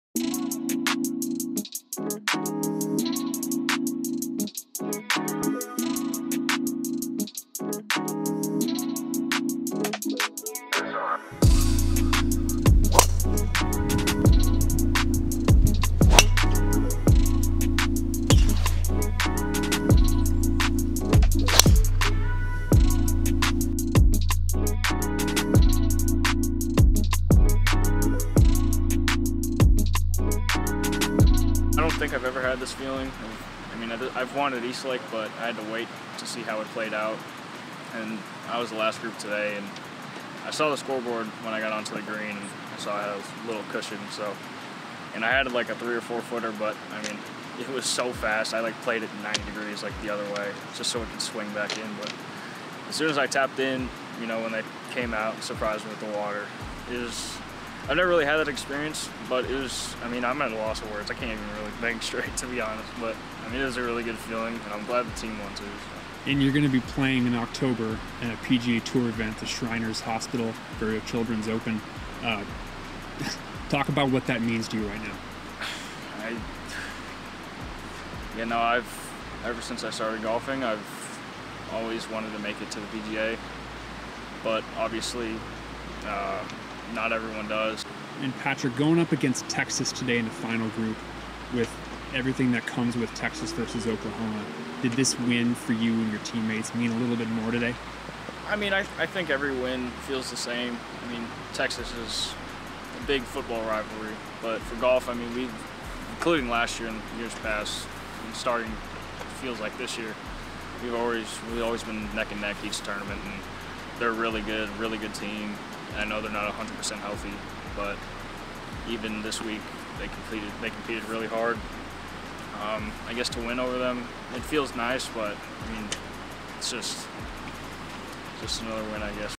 2022 Southern Highlands Collegiate Highlights
AmateurGolf.com interviews the individual winner Patrick Welch and takes you inside Las Vegas' Southern Highlands Golf Club, the host course of the Southern Highlands Collegiate